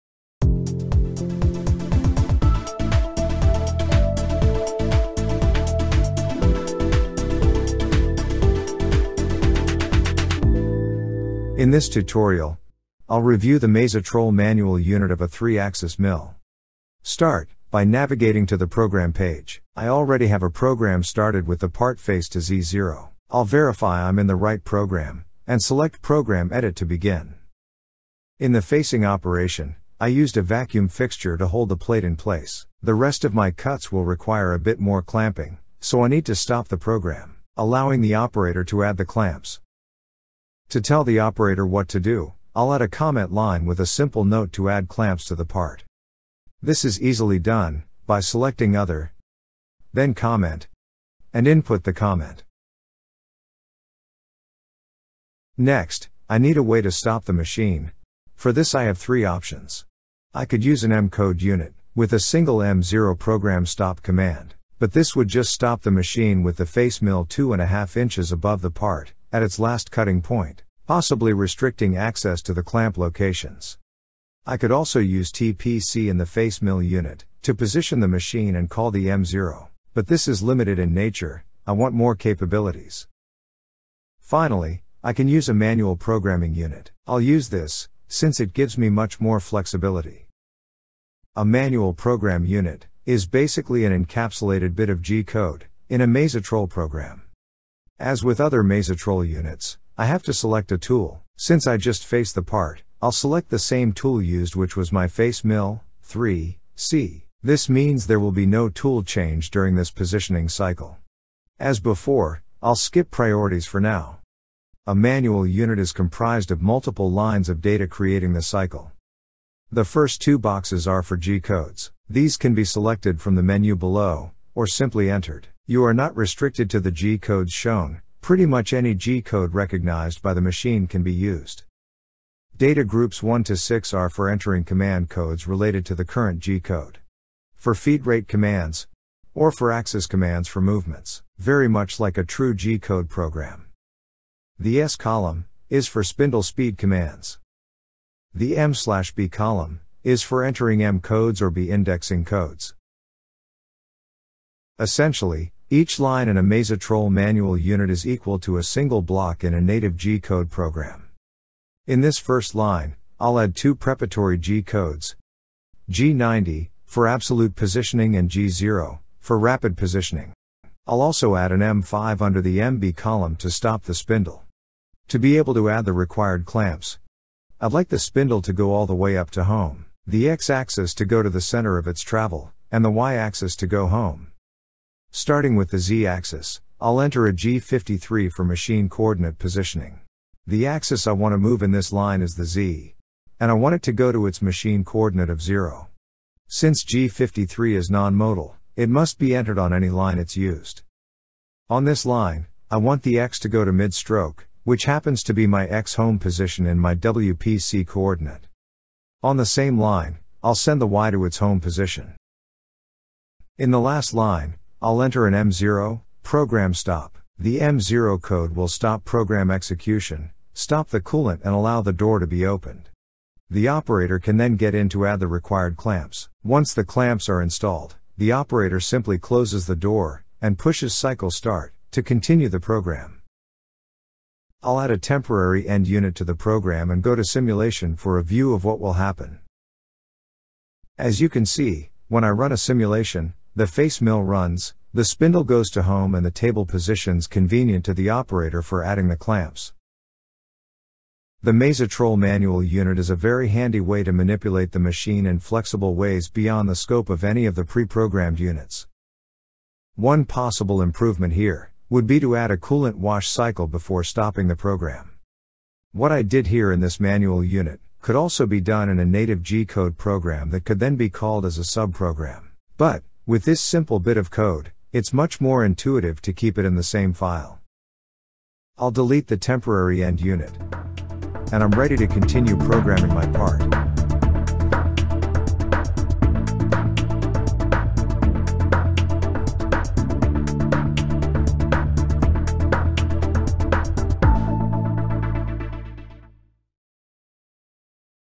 MAZATROL Programming Briefs - Manual Programming Unit
This tutorial reviews the MAZATROL Manual Unit of a 3-axis mill.